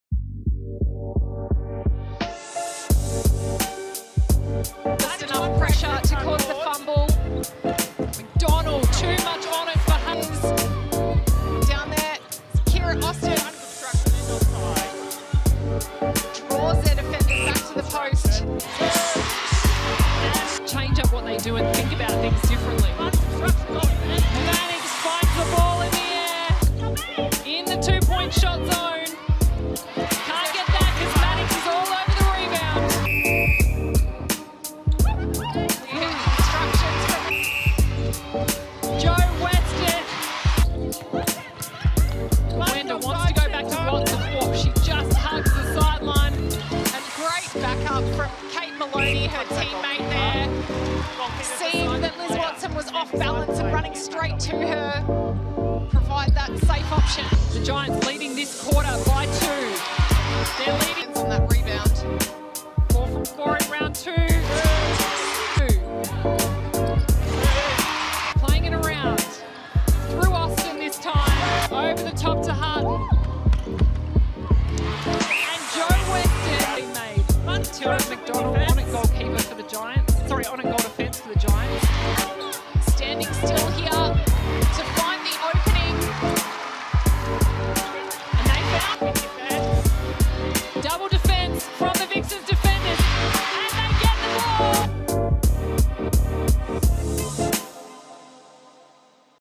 Moments that Mattered - Round 3
Catch all the highlights from our first home game against the GWS Giants Netball at John Cain Arena.